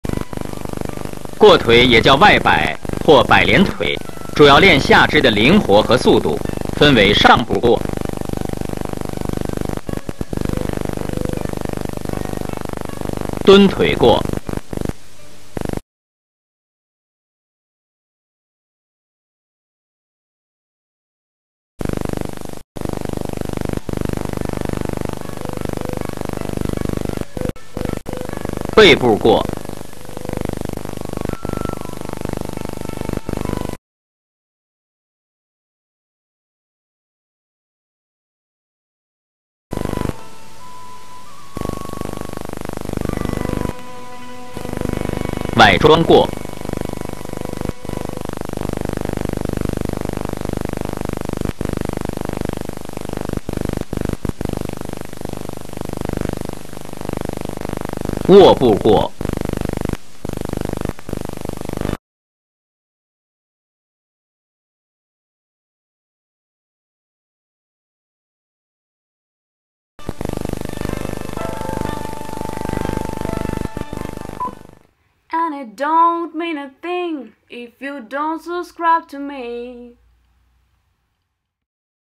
过腿 GUO TUI and ASIAN SQUAT
The Guo Tui technique, also called Wai Bei Tui, Straight Out Leg, is a very common leg technique, both in wrestling and other martial arts styles. But in wrestling it's used to avoid a take down, while in other styles it's a foot strike. The technique and its concepts are however the same, indeed, as you can see in this video the wrestlers perform it using some famous "Asian Squats". Squats that have been talked about a lot lately for their health benefits, but which in the East are simply part of tradition and everyday life. Performing it as wrestlers do it is invaluable to all martial arts practitioners.
At the end of the video a link to my video where I explain the many uses of this technique as a foot strike.
Shooting of the school of the Master Li Baoru, Beijing, eighties.

IT La tecnica Guo Tui, chiamata anche Wai Bei Tui, Gamba tesa verso l'esterno, è un tecnica molto comune, sia nella lotta tradizionale che in altri stili di arti marziali. Ma nella lotta viene utilizzata per evitare un proiezione, mentre negli altri stili è un colpo con il piede. La tecnica e i suoi concetti sono comunque gli stessi, anzi, come si vede il questo video i lottatori la eseguono utilizzando alcuni famosi "Asian Squat". Squat di cui ultimamente si sta molto parlando per i loro benefici sulla salute, ma che in oriente fanno semplicemente parte della tradizione e della quotidianità.
A fine video un link ad un mio video dove spiego i molti usi di questa tecnica come colpo di piede.
Riprese della scuola del Maestro Li Baoru, Pechino, anni ottanta.